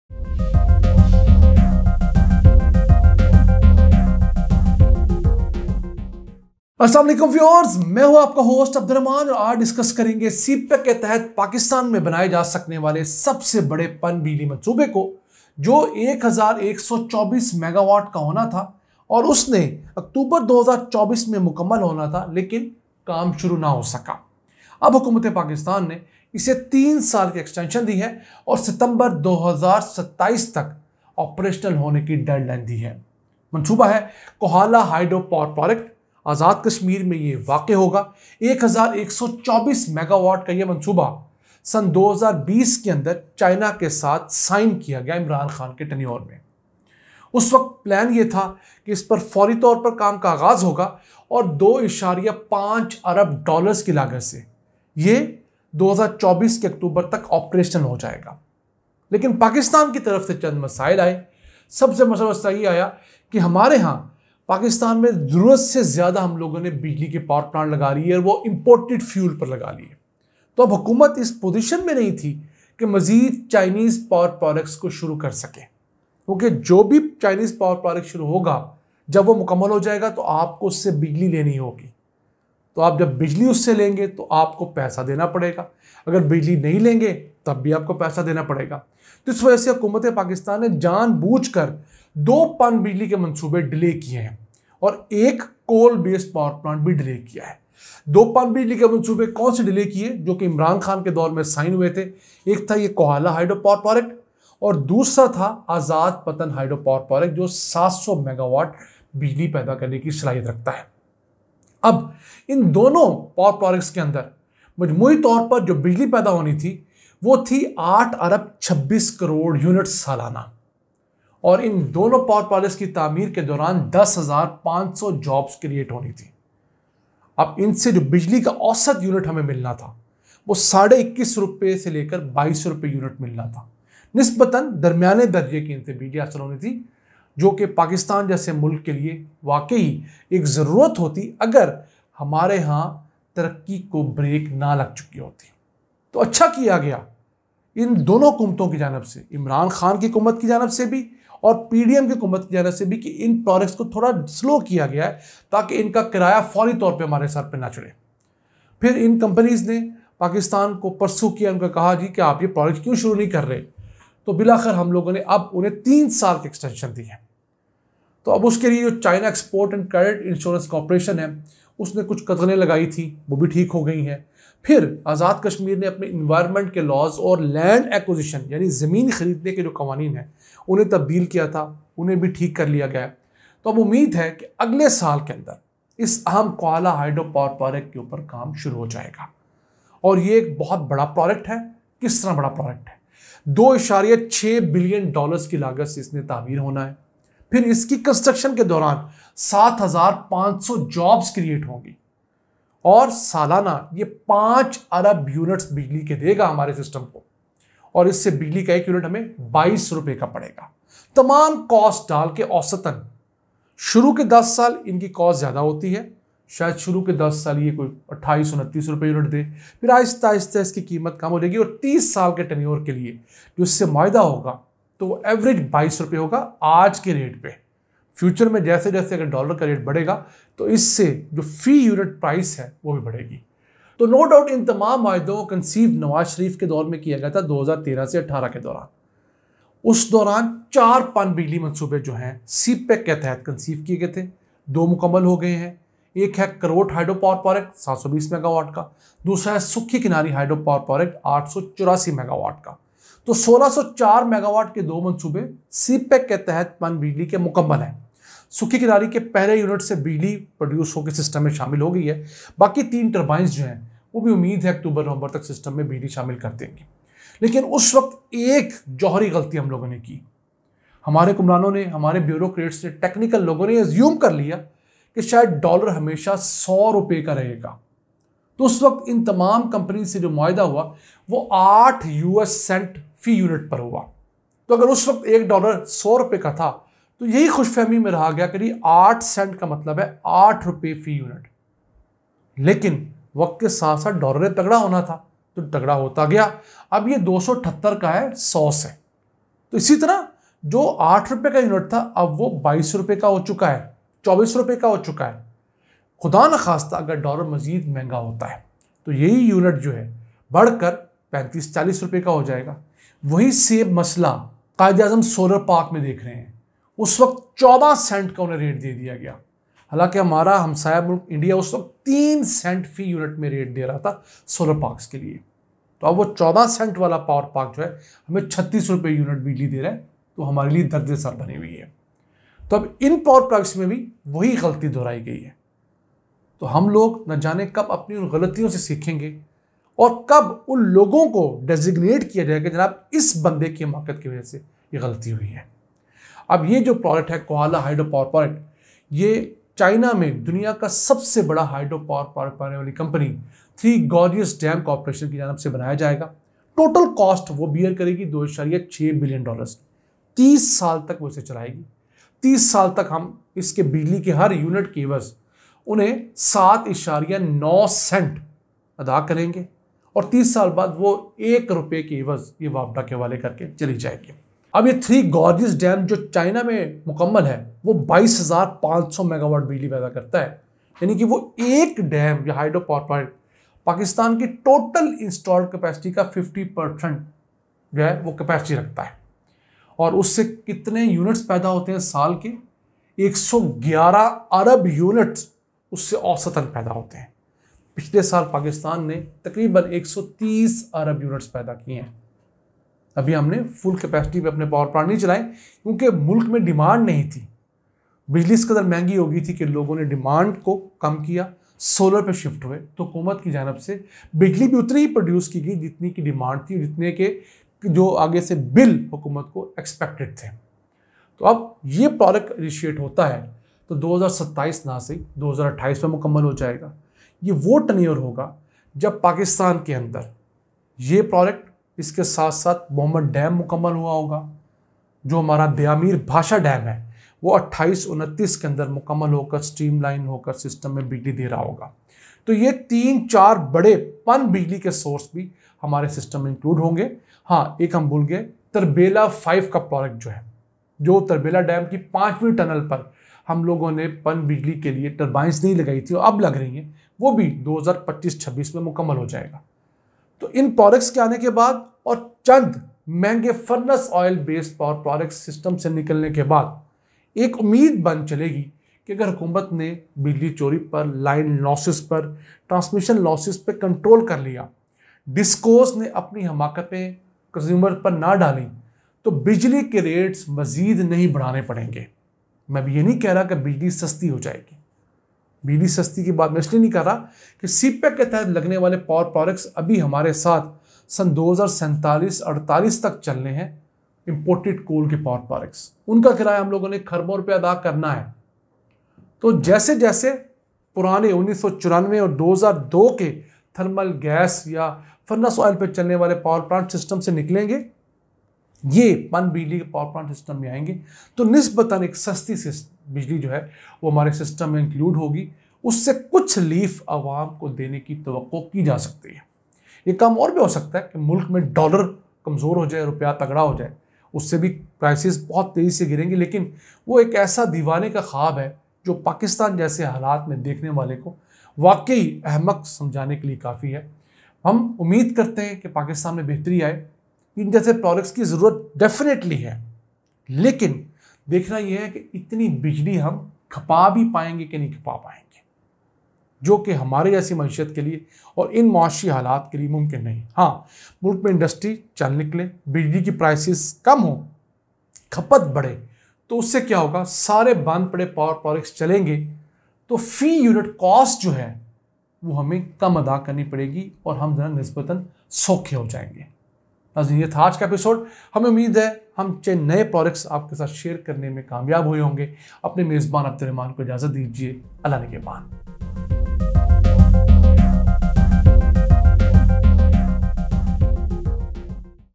World Largest Hydro Power Project Company will construct Kohala Hydro Project in Pakistan
(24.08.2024) Kohala Hydro Power Project 1124MW will be constructed by World's Largest Hydro Power Project Three Gorges Dam company.This project will generate 5 Billion units electricity annually.